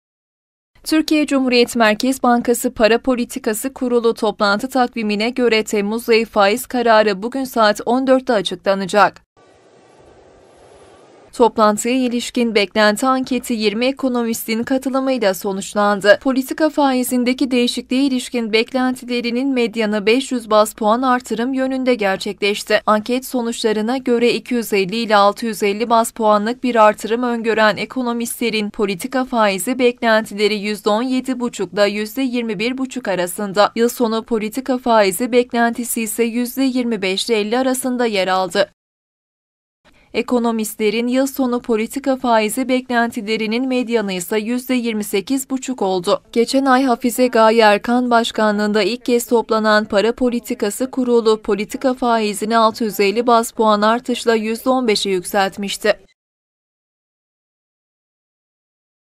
GÖZLER MERKEZ BANKASI'NDA
Merkez Bankası para politikası genel kurulu faiz kararını bugün açıklayacak